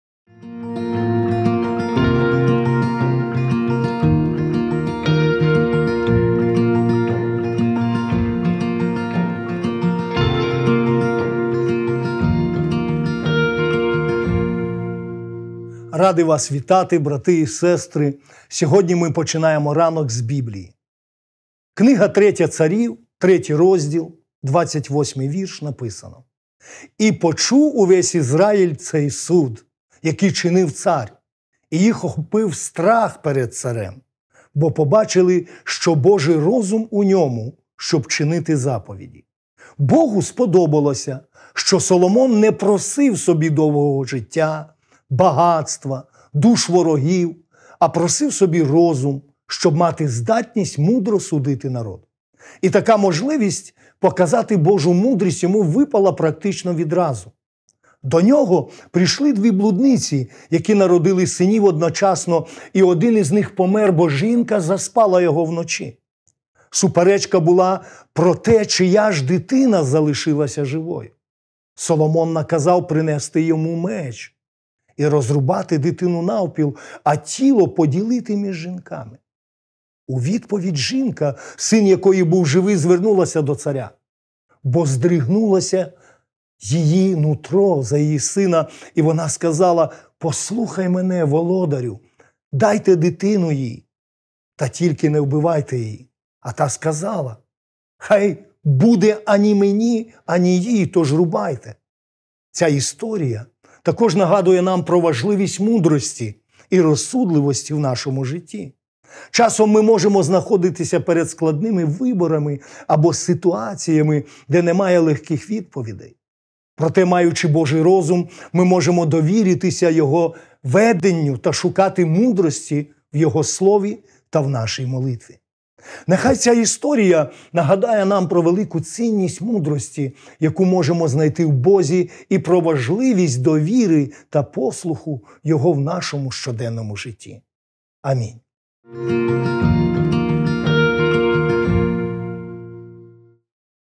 «Щоденний хліб» / 25 квітня / Леонід Тимощук
«І почув увесь Ізраїль цей суд, який чинив цар, і їх охопив страх перед царем, бо побачили, що Божий розум у ньому, щоб чинити заповіді» 3 Царів 3:28 (UTT)